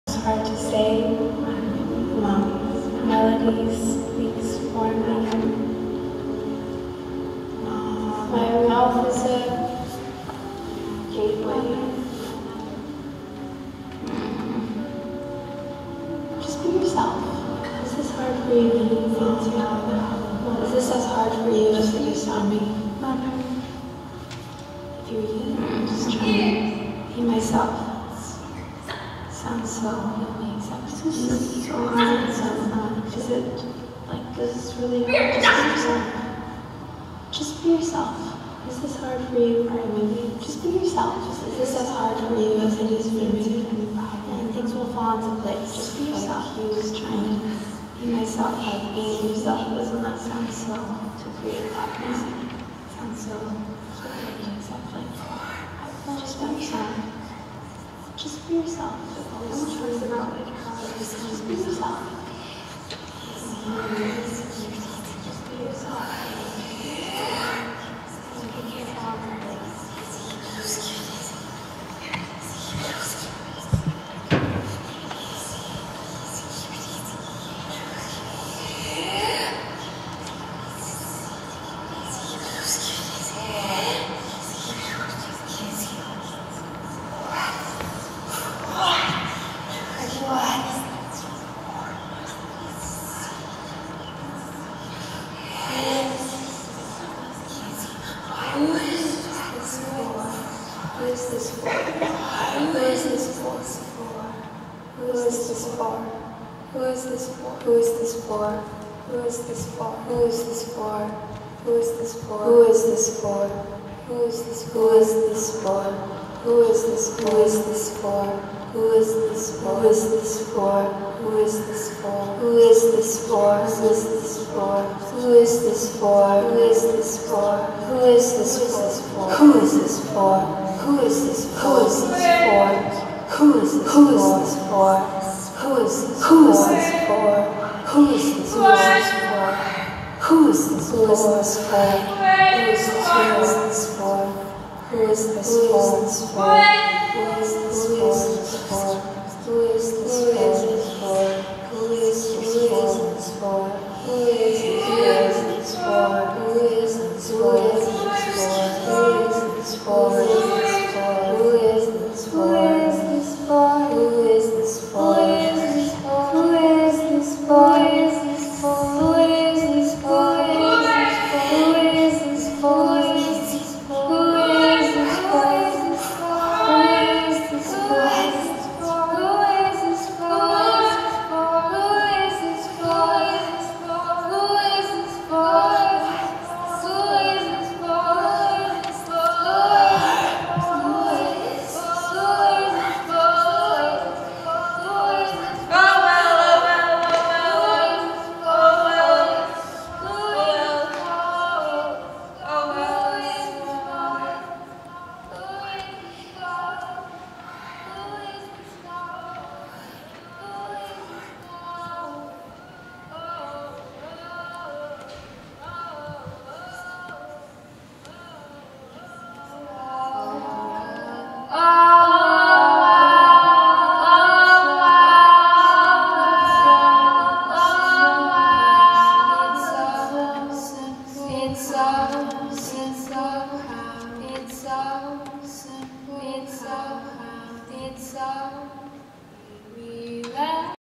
Laurel Snyder - Movement Research at the Judson Church - March 2, 2020
To What End (study #11)
Created and performed by Laurel Snyder

Videography by Carley Santori
Movement Research at the Judson Church